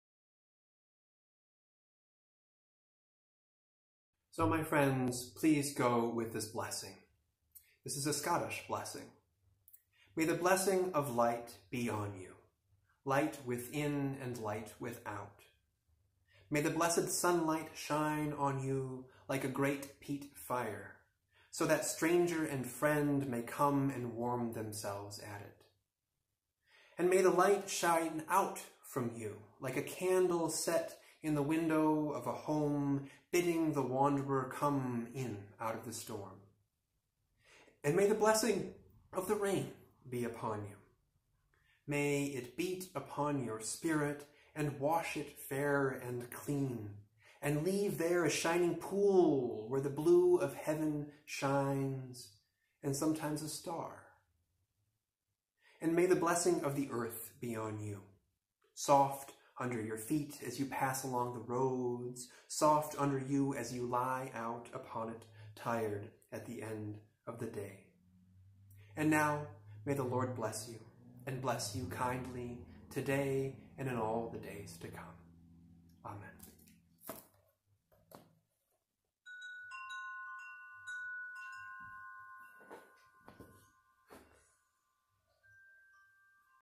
Benediction
May the blessing of light be on you - light without and light within.
May the blessed sunlight shine on you like a great peat fire,
so that stranger and friend may come and warm themselves at it.
And may light shine out of the two eyes of you,
like a candle set in the window of a house,
bidding the wanderer come in out of the storm.
And may the blessing of the rain be on you,
may it beat upon your Spirit and wash it fair and clean,
and leave there a shining pool where the blue of Heaven shines,
and sometimes a star.
And may the blessing of the earth be on you,
soft under your feet as you pass along the roads,
soft under you as you lie out on it, tired at the end of day
And now may the Lord bless you, and bless you kindly. Amen.
-Scottish Blessing